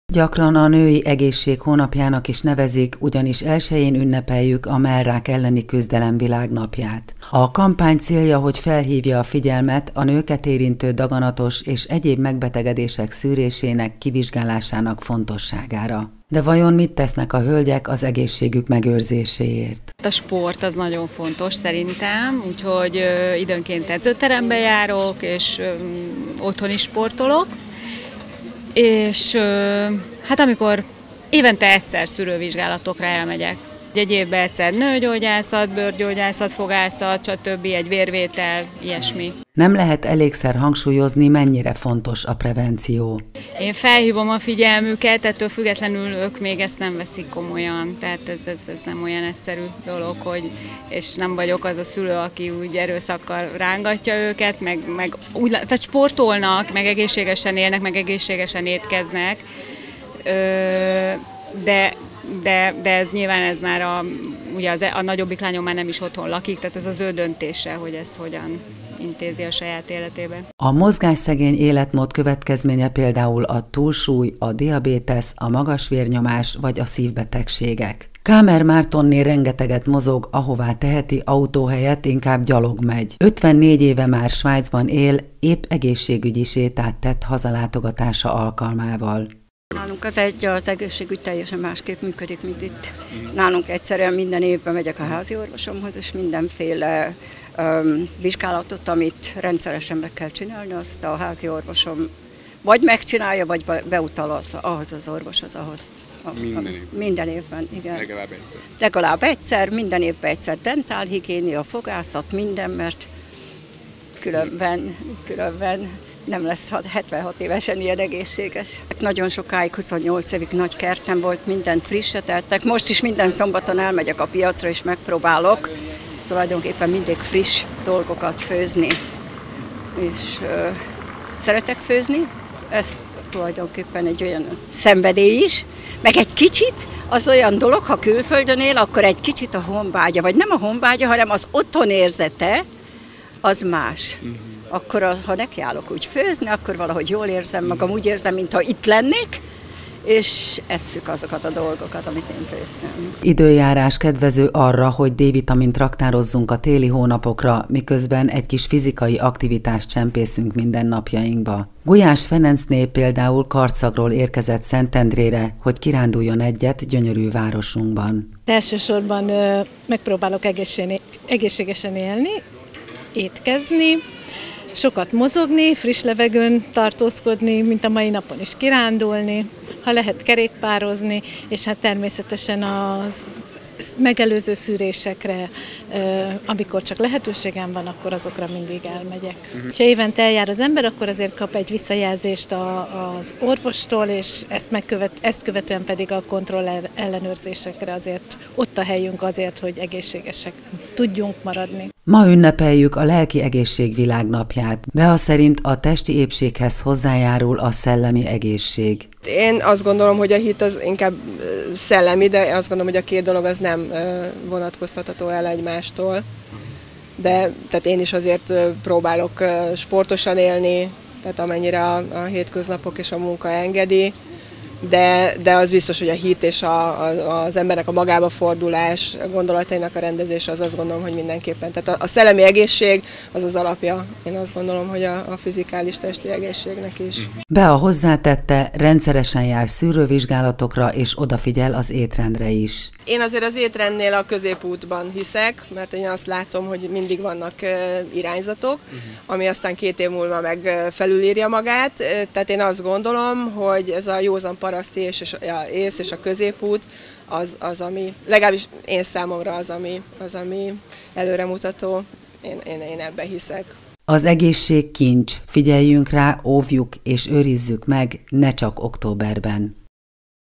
Megkérdeztük a hölgyeket, ki mennyire figyel oda testi - lelki épségére /Szentendre MA/ 2022.10.10.
Egészség - megkérdeztük a hölgyeket, ki mennyire figyel oda testi, valamint lelki épségére